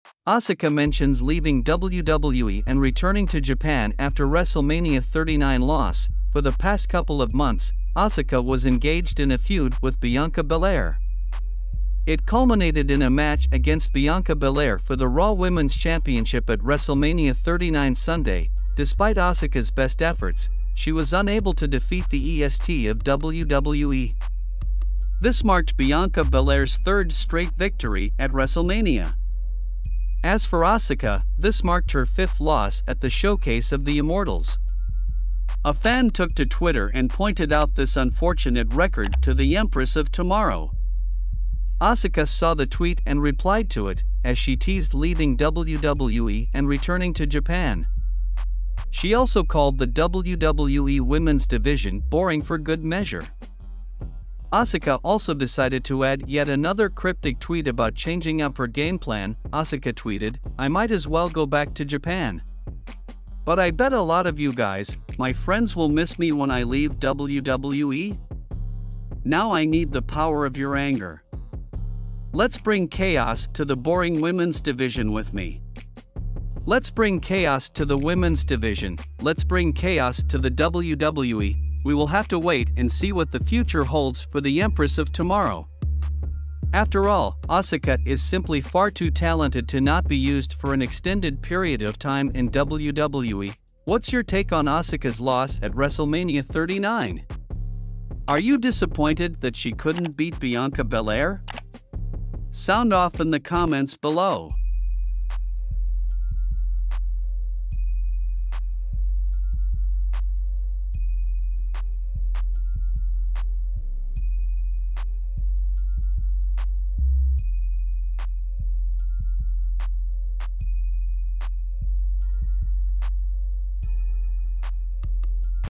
Asuka Mentions Leaving WWE & Returning To Japan After WrestleMania 39 Loss
WWE Star Calls Women's Division 'Boring', Says 'A Lot Of You Will Miss Me When I Leave'
Asuka On WrestleMania 39 Loss – ‘I Might As Well Go Back To Japan’